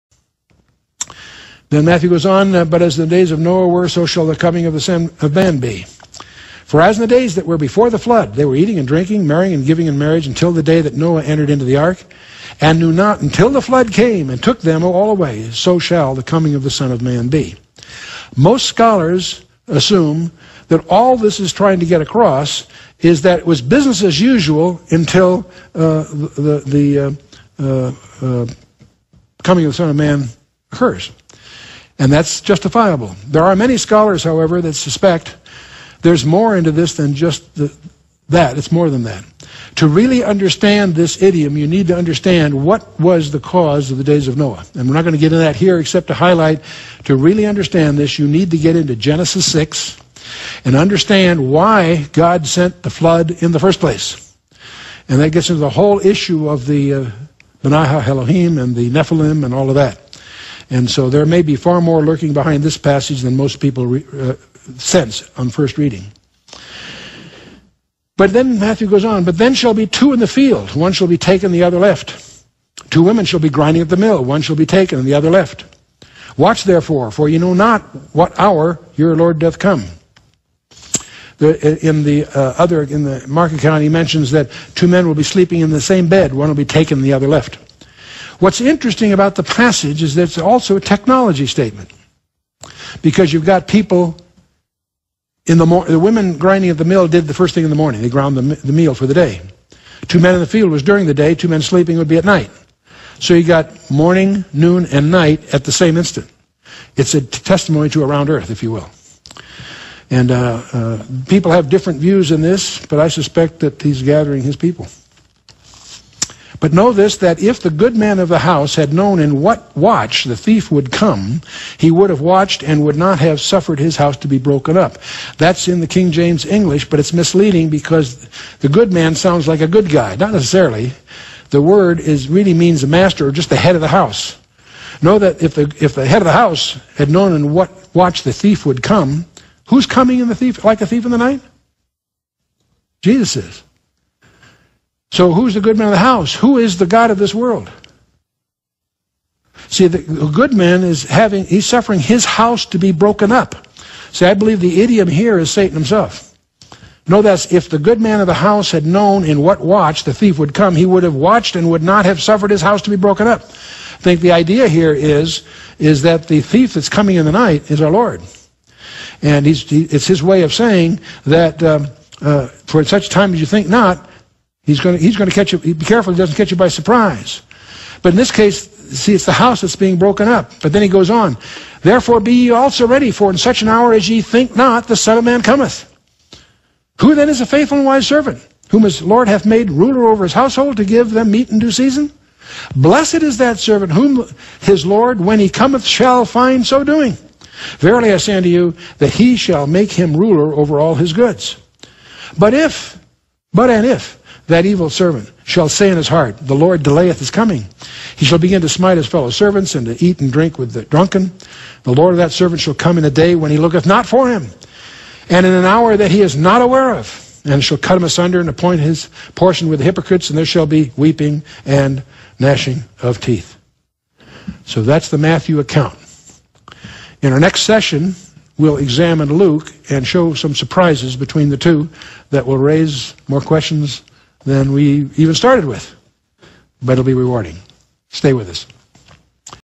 Round Earth (Flat False) Scripture Proof, Pre Trib Rapture, Chuck Missler Matthew 24
Chuck Missler gives us a Pre Trib Rapture, round earth, explanation.